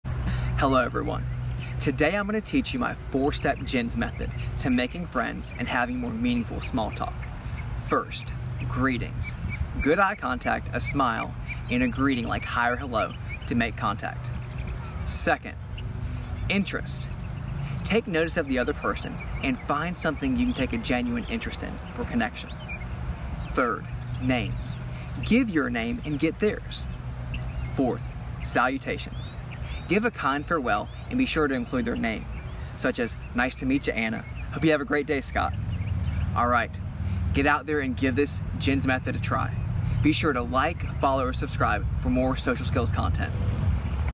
The G I N S Method Medium
Scott Miller talks about the four step G.I.N.S. Method to making friends